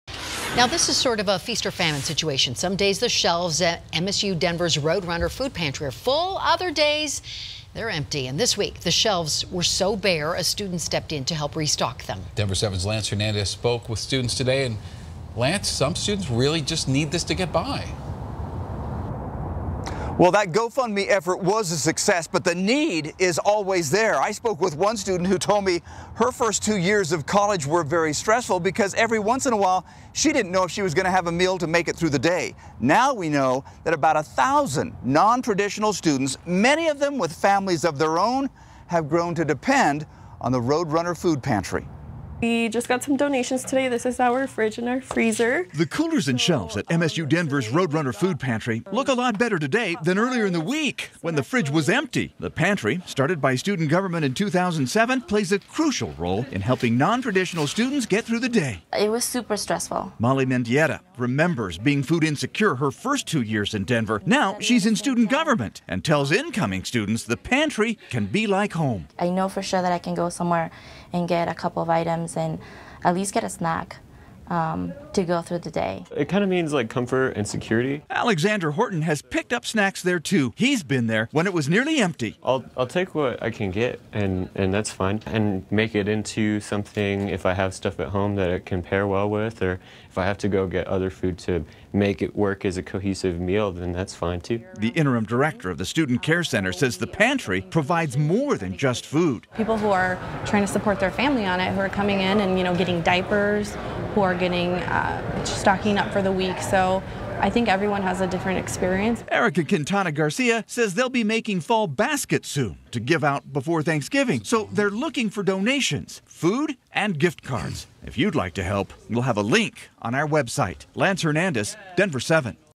GoFundMe account set up to help restock MSU Denver's Roadrunner Food Pantry
It's feast or famine at the Roadrunner Food Pantry. Some days the shelves are full, some days they're nearly empty.